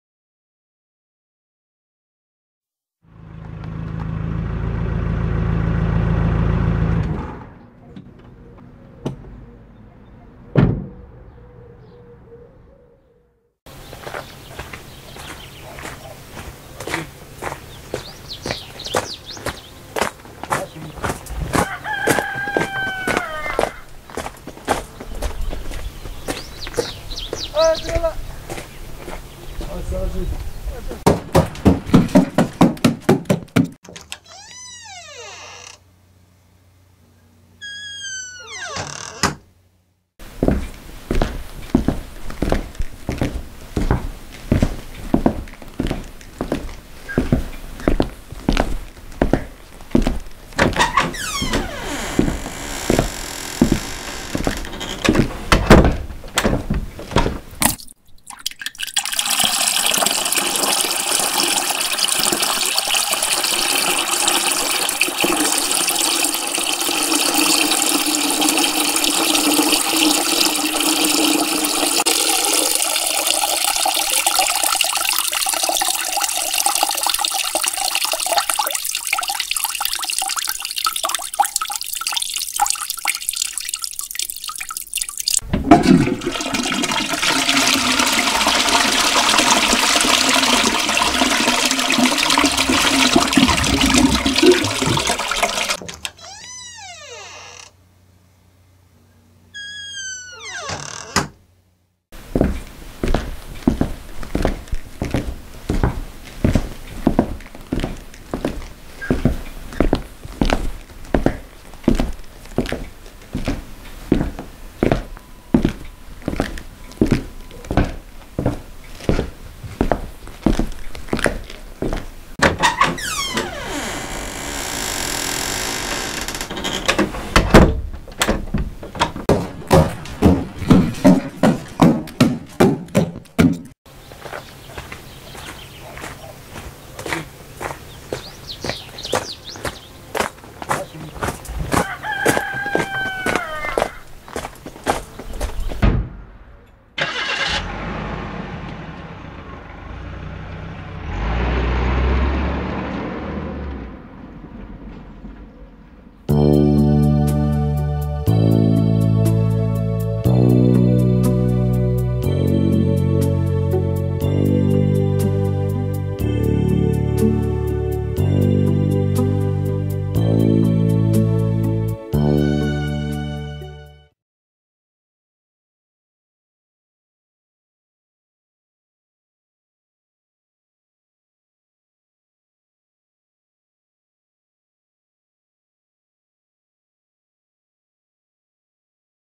UrgentBusiness3min9sec
This project involves no actors, and no talking, just  sound bites and clip art, all free from the internet. Credit for sound bites is to FreeSound.org and their many talented contributors. This video is dedicated to all senior males of the world.